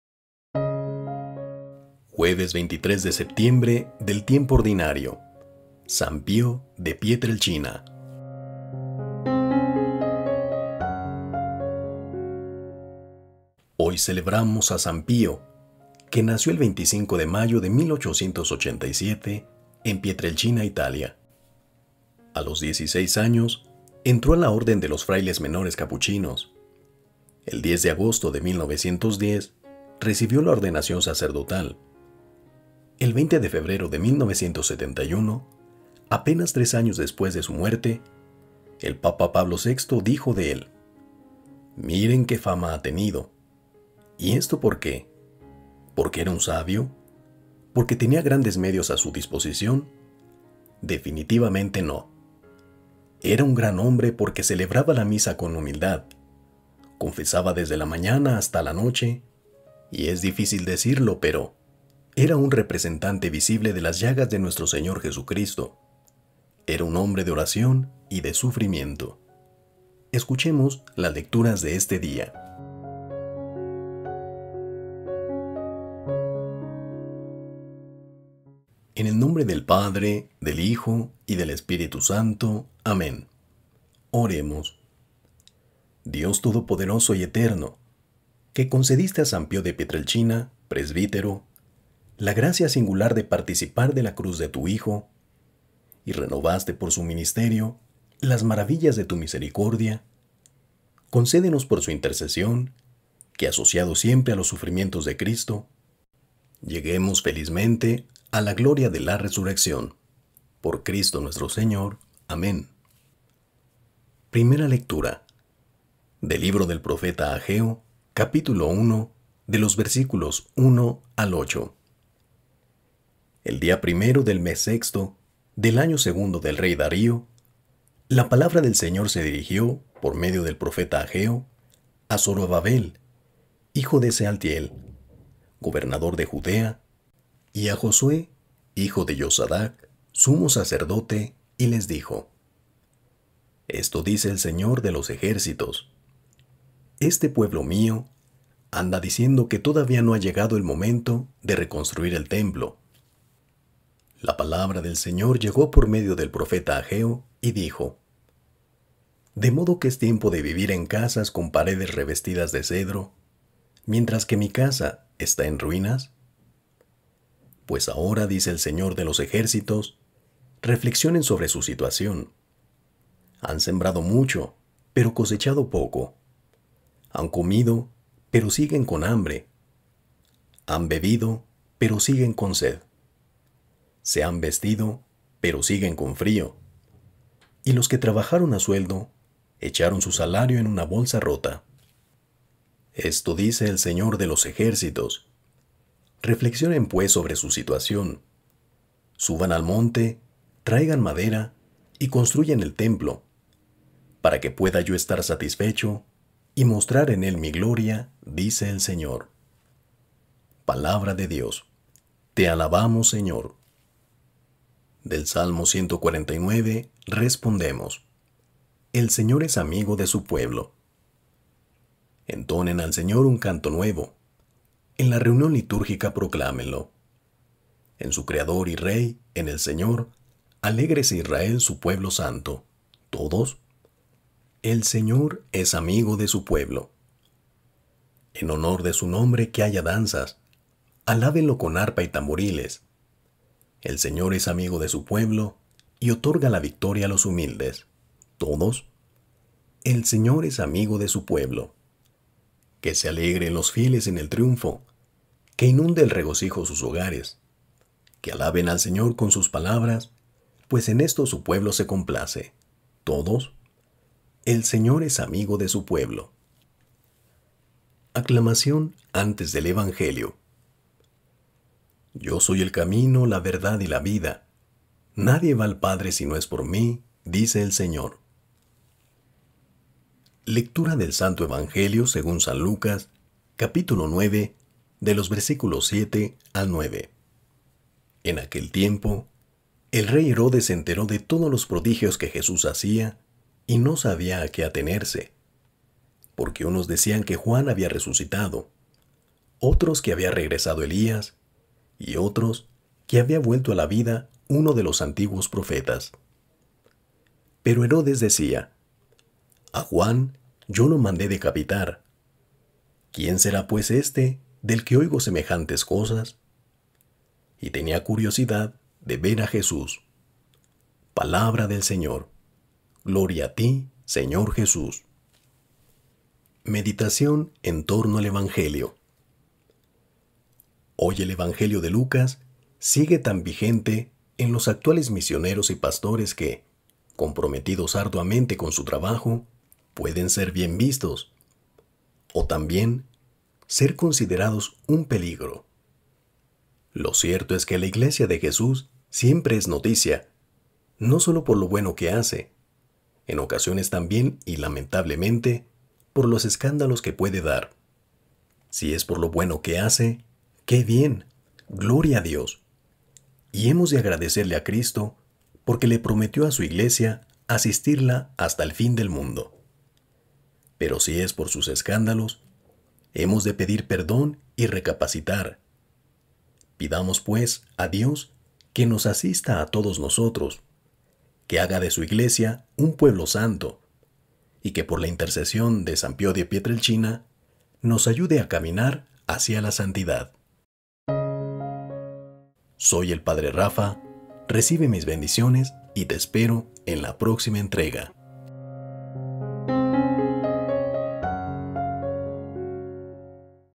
Evangelio del día: Jueves 23 de septiembre de 2021
En aquel tiempo, el rey Herodes se enteró de todos los prodigios que Jesús hacía y no sabía a qué atenerse, porque unos decían que Juan había resucitado; otros, que había regresado Elías, y otros, que había vuelto a la vida uno de los antiguos profetas (Lc 9,7-9).